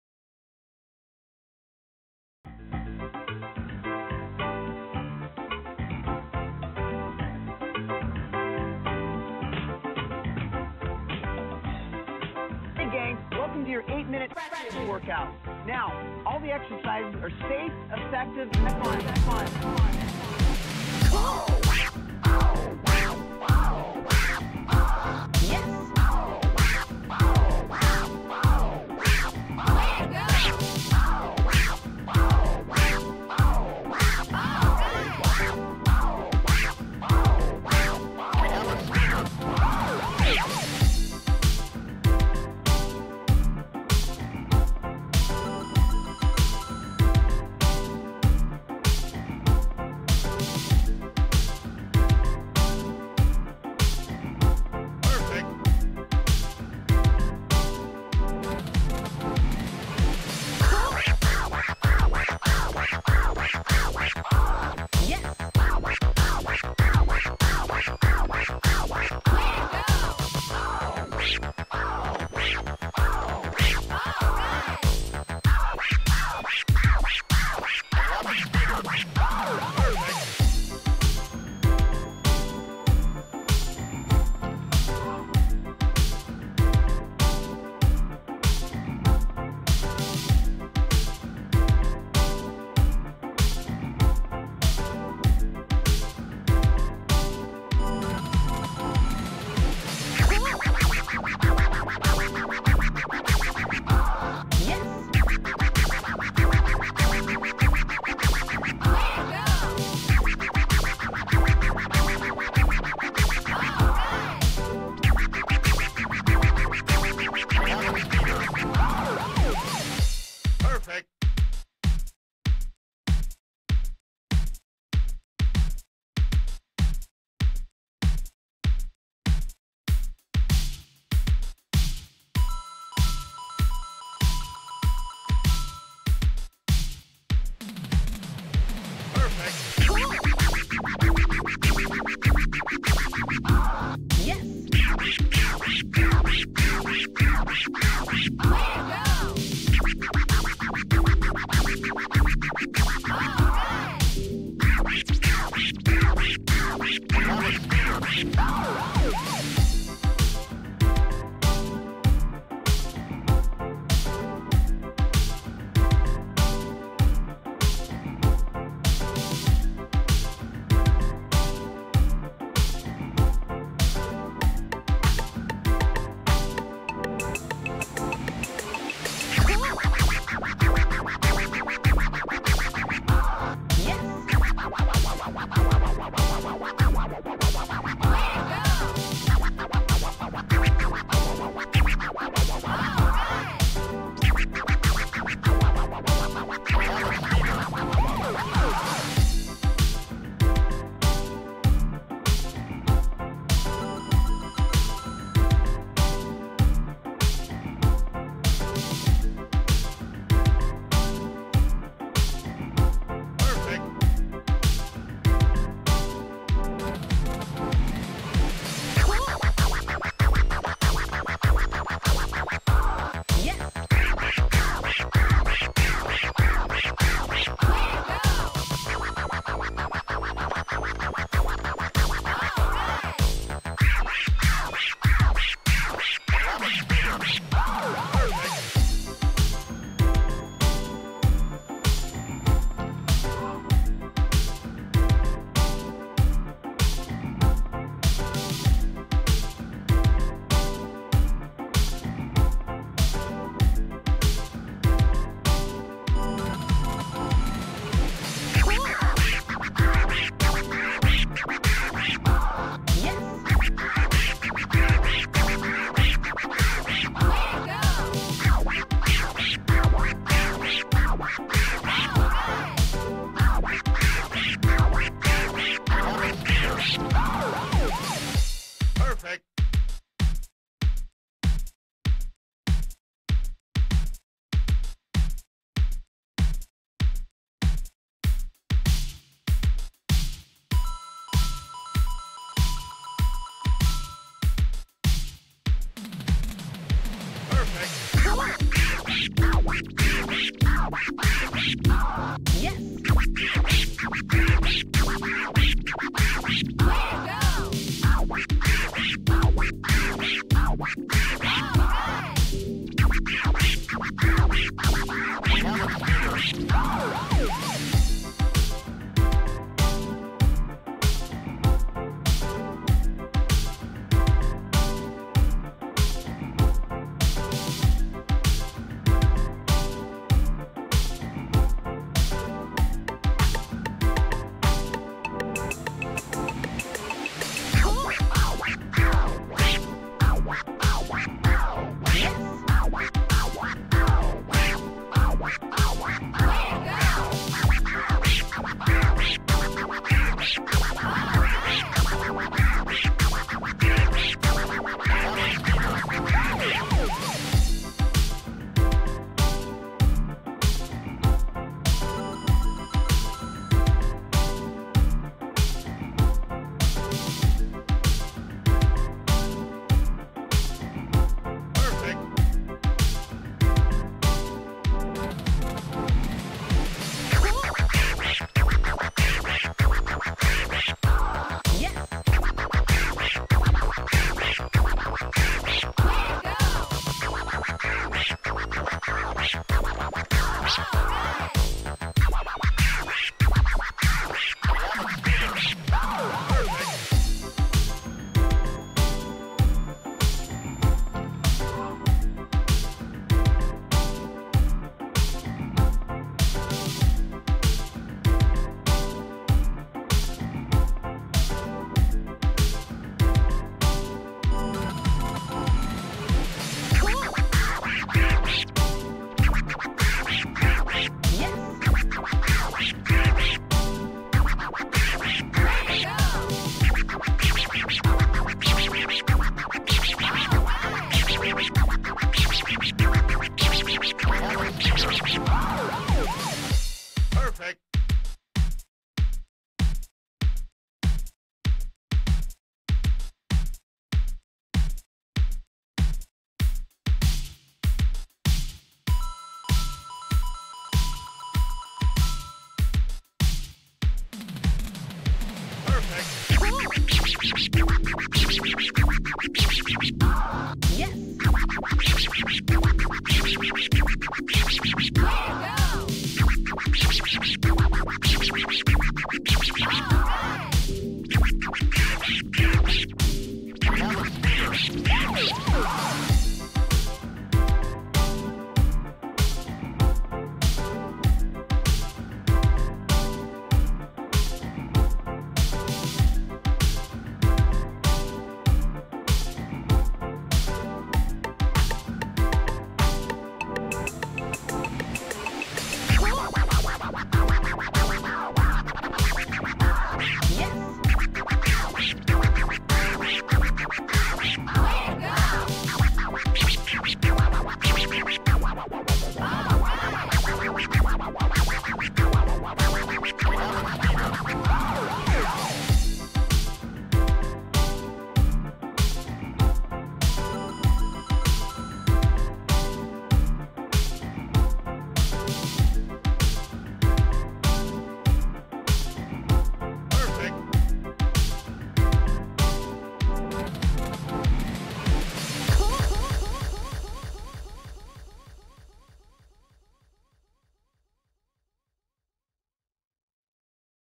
8 MIN SCRATCH WORKOUT I BABY SCRATCH LVL1
8 min Abs-solute scratch workout is a great workout or warmup for both beginner and experienced Djs!

8 min Abs-solute scratch workout series will be updated with new scratches and new levels of difficulty and focused scratches to keep you working and motivated to get better. Repeat this and other workouts everyday with left then right hand and track your improvements.

You will also notice that learning newer & harder scratches will be easier for you when you have consistently practiced your foundation cuts with skratchercise workouts like this!

Getting better takes time. But even a few mins a day helps!

This episode is a Level 1 Baby Scratch focused session!

Use an Rca to Stereo Cable and plug into your Line-in or Aux-in on your mixer with the other side going into your phone or laptop to follow along!

Dj will demonstrate focused scratch for 8 bars or 32 beats. then you have 8 bars or 32 beats to focus on the same scratch!

Beat by: Ted-D (ig @djteddmusic)
Sample: 8 Min Abs Routine
Instructor: Dj Relik (ig @djrelik)

Equipment used:

NEEDLES - Shure M44-7 - Discontinued